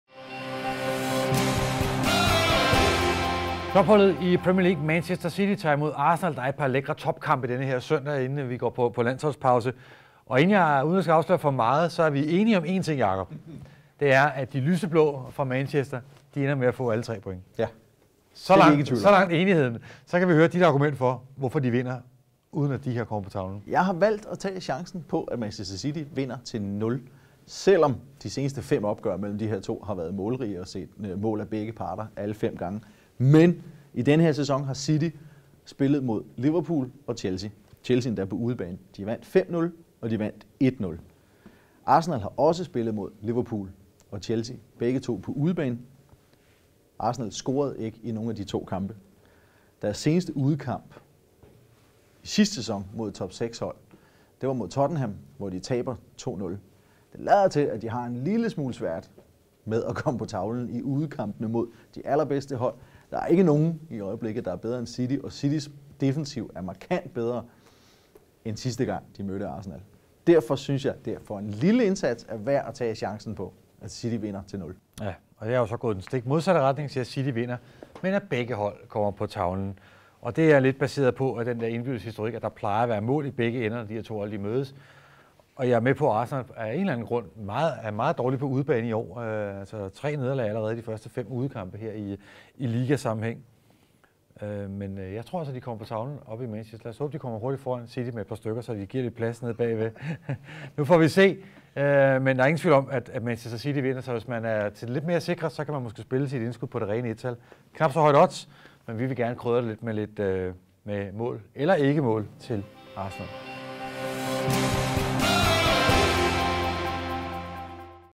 Manchesters målmaskine moser Arsenal!
City har i snit leveret 4,2 mål hjemme på Etihad i denne sæson. Det er ubehagelig læsning for Wengers mandskab, der kun har snittet 1,2 scoringer på udebane. Jacob har derfor heller ikke megen fidus til andet end favoritsejren, men vælger at booste sit odds i jagten på bedre værdi. Se hvordan lige her.